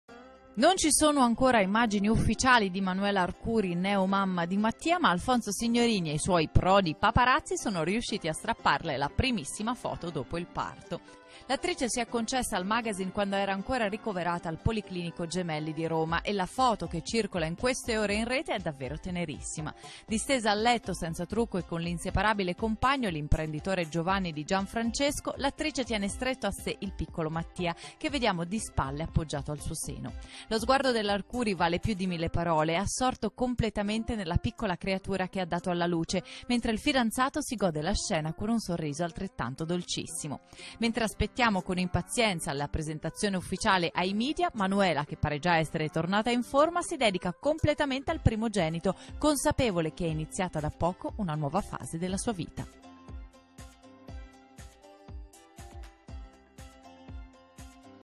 La prima foto di Manuela Arcuri e Mattia
L'attrice si è concessa a 'Chi' di Alfonso Signorini